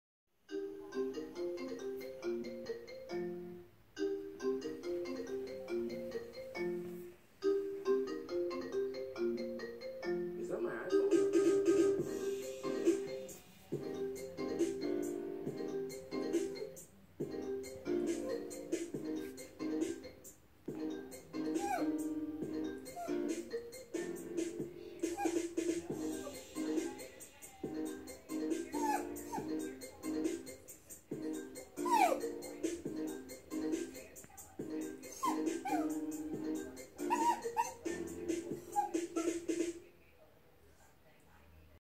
Dog confused by iphone ringtone
When you can hear your ringtone but can't find your phone !